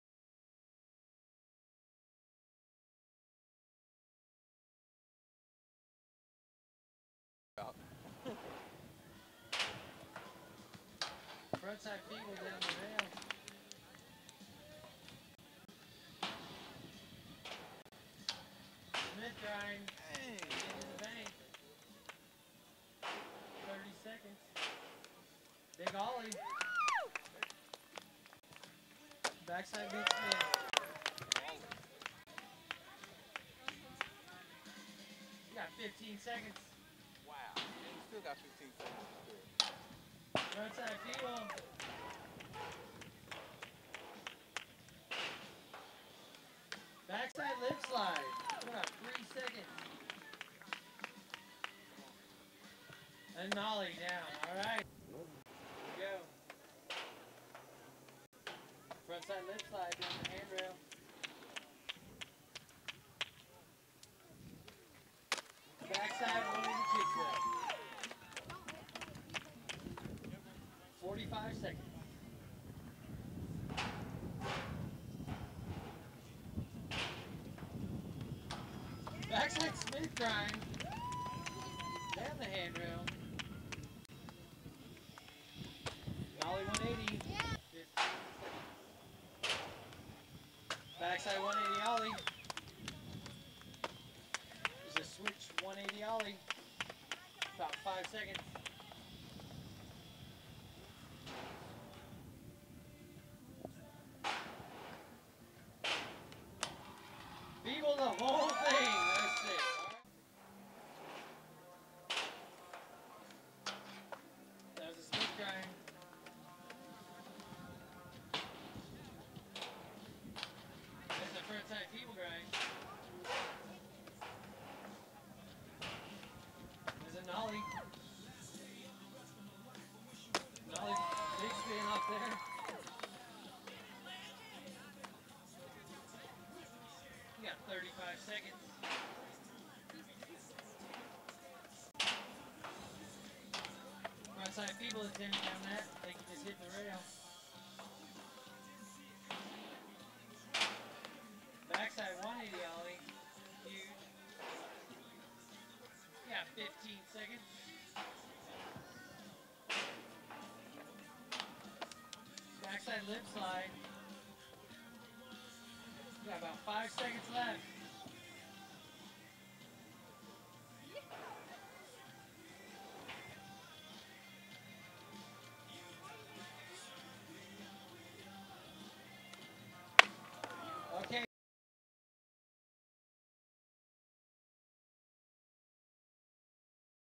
Ryan Woodward skate comp 2011, Landon Barnhart 1st place runs in advanced
i got first place!! ,, these are my runs from the Ryan woodward skate comp 2011,  i only fell on ONE of my tricks in the whole comp!  i landed every single other trick,   i'm so excited about winning the comp!    filmed by my dad ; Breck Barnhart  thanks for watching,     R.I.P Ryan Woodward   skate4ryan.com